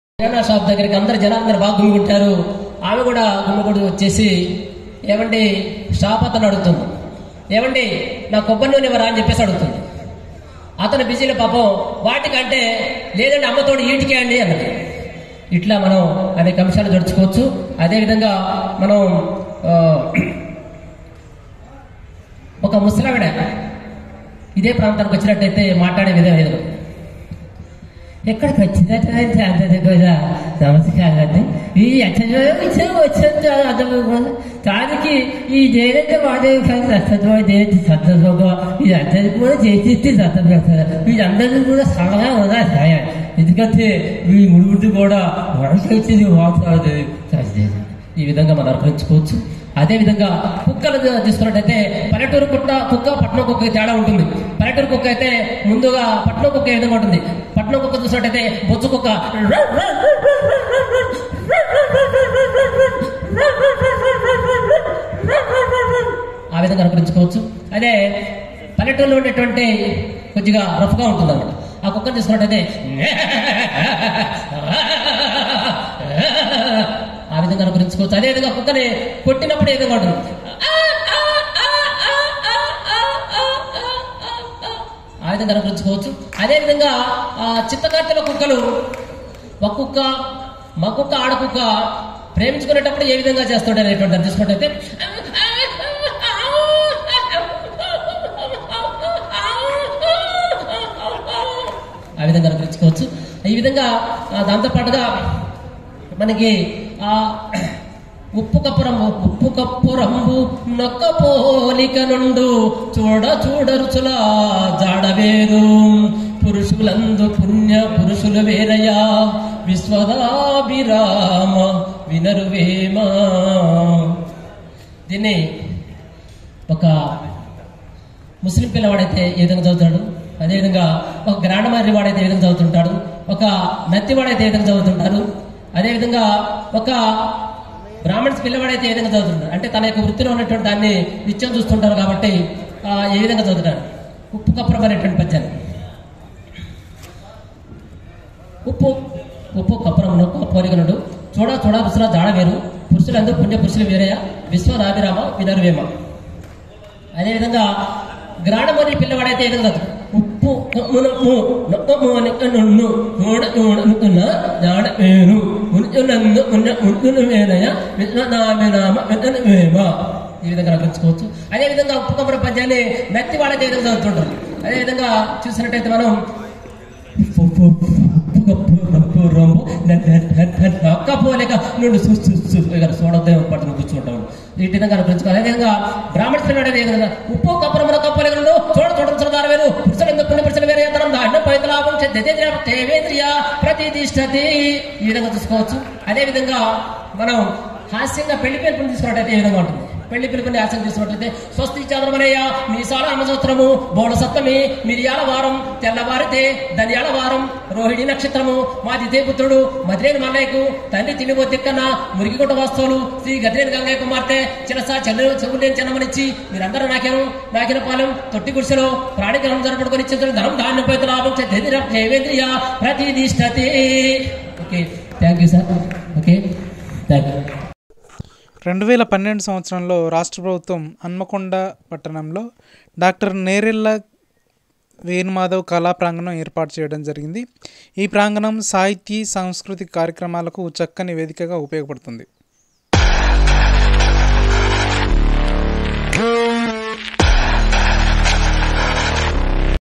Mimicri Artist live performance in Nerrella Venumadhav birthday & Awards function
In memory of legendary mimicry Artist -Nerella Venumadhav birthday function